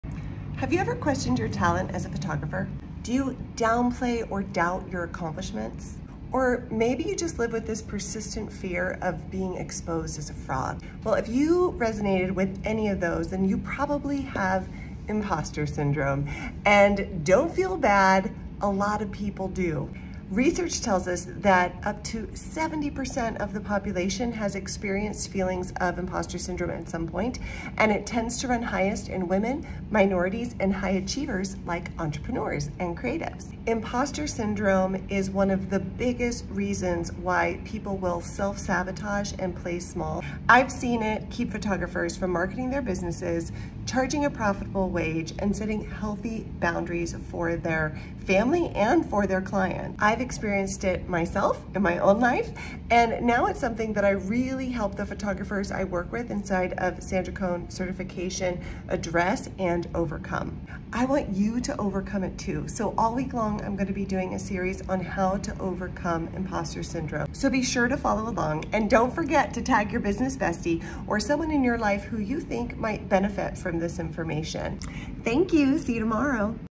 Imposter Syndrome Series: Part 1
Have you ever questioned your talent as a photographer?

Do you downplay or doubt your accomplishments?

Or maybe you live with a persistent fear of being exposed as a "fraud”?

Well that my friend, is Imposter Syndrome.

And it’s incredibly common!

An estimated 70% of the population as experienced it at some point in their lives.  But it tends to to run highest in woman, minorities, and high achievers, like entrepreneurs.

Imposter Syndrome is one of the biggest reason why people self sabotage and play small.

I’ve seen it keep photographers from marketing, charging a profitable wage, and setting healthy boundaries in their business.

I’ve experienced it in my own life and it’s something I now help other photographers overcome it inside of Sandra Coan Certification.

I want to help you over come it to!  So all week long, I’m going to be sharing tips for overcoming imposter syndrome!

Be sure to follow along!  And don’t forget to share this with your business bestie, because (spoiler) talking about this with others is one of the ways to overcome it!